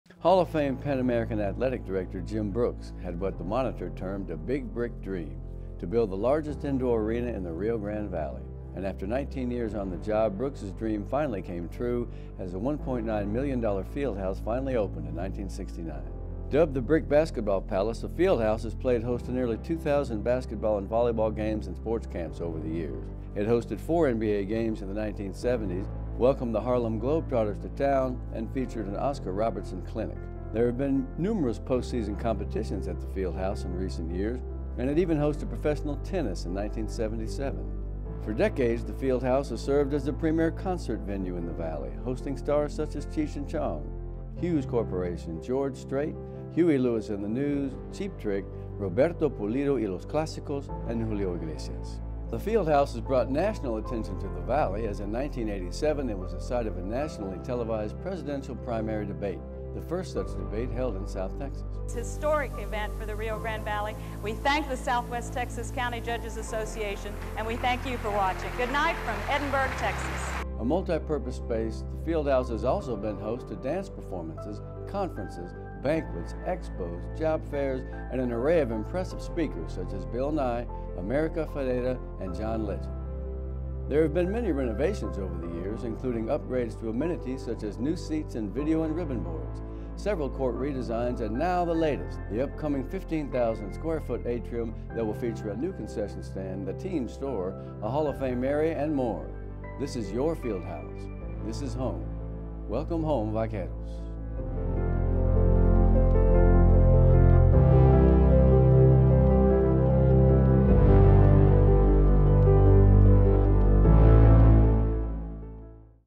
UTRGV Athletics Expands on Vision of First Athletic Director with Enhancements to UTRGV Fieldhouse
GoUTRGV.com

RIO GRANDE VALLEY, Texas – During his time as athletic director from 1952-73, hall of famer Jim Brooks oversaw the construction of the current UTRGV Fieldhouse and health and physical education complex, Orville Cox Tennis Center, and Jody Ramsey Memorial Stadium, as well as many other advancements throughout the athletic program.

UTRGV is currently amid more than $85 million in facilities enhancements affecting all 18 sports, including recently completed upgrades to the inside of the UTRGV Fieldhouse, which was built in 1969, such as new ribbon and video boards, new seating, and a redesigned court.

The construction of a brand new 15,000-square foot atrium with a lobby that will include a new entrance, new box office, new, unique way of watching games at the baseline club, a designated area for the hall of fame and hall of honor, a team store, new concessions, and new restrooms, is expected to be complete in the spring of 2025.

Jim Brooks was initially hired as the head football coach at Edinburg Junior College (EJC), the predecessor to UTRGV legacy institution Pan American College (PAC), in 1950, coaching the team’s final season before becoming the department chair of health and physical education during the transition to becoming a baccalaureate college. Brooks was then hired as the first athletic director in university history, spending 31 years in that role and as athletic director emeritus (1973-83) at PAC and Pan American University (PAU).

His first major accomplishment came when he convinced the administration to transform an old bus barn into a multi-purpose gymnasium.

When the fieldhouse opened, an article in The Edinburg Daily Review stated that “the old gym has a seating capacity of zero and a usage factor of about 200 percent, since it is used day and night for physical education classes, basketball practices, intramurals, and other activities.”

That old gym sat on a seven-acre patch of land that the university owned about two blocks west of the main campus of EJC and the early years of PAC, where the current Edinburg City Hall is located.

“I remember when all this was a seven-acre carrot patch,” Brooks told the Valley Morning Star in 1969. “In the summer it was seven acres of sticker burrs. It was just a small, L-shaped patch of land the college owned, west of the main campus, and the college had somebody farm it for them.”

The seven acres turned into what was popularly known as “West Campus,” which hosted the gymnasium, a track & field, and all the physical education classes for the university at the time. This area still hosts a track & field and physical education classes for UTRGV students today, as health and physical education II is located on that land.

For 17 years, PAC practiced at that gym, played at Edinburg High School, and found unprecedented success highlighted by winning the 1963 NAIA National Championship in Kansas City, Mo. Before returning home, Brooks was told to stop the bus in Austin for a celebratory event for the team.

The bus was given a police escort to the state capitol and the team received a commendation in front of the state legislature. Three days later, PAC was given full state support by the legislature.

In 1965, Texas Proposition 1 provided infrastructure upgrades for 17 higher education institutions, including PAC, which allowed for the construction of the current UTRGV Fieldhouse and health and physical education complex I.

This monumental facility was completed in 1969 and instantly became a cornerstone on campus, hosting all volleyball and men’s and women’s basketball home contests, as well as other major university events like graduations, career days, and registration.

The fieldhouse has served as a premiere entertainment venue for the entire Rio Grande Valley, hosting four NBA games in the 1970s, musicians among the likes of country music icon George Strait, GRAMMY award-winners Huey Lewis and the News and Julio Iglesias, and a nationally televised presidential primary debate in 1987, the first of its kind in the Valley.

The fieldhouse continues to be the culmination of the vision that Brooks had for the university. From convincing the administration to allow him to host classes on the current UTRGV Edinburg Campus, to receiving nationwide attention in multiple sports, and to helping the university receive state support, Brooks was instrumental in transforming EJC into PAC and PAU, leading to membership in the UT System in 1989 as UT Pan American and now as UT Rio Grande Valley.

“[Jim] Brooks had the vision to see Pan American as it is now,” UTRGV hall of fame men’s basketball coach Sam Williams told Mario J. Reyna for his thesis in 1988. “I think because of Brooks’ stand, our programs are the way they are today.”

Today, UTRGV celebrates the work of former athletic director and professor Jim Brooks for his work towards the university and is expanding on his vision with the enhancements being made to the UTRGV Fieldhouse.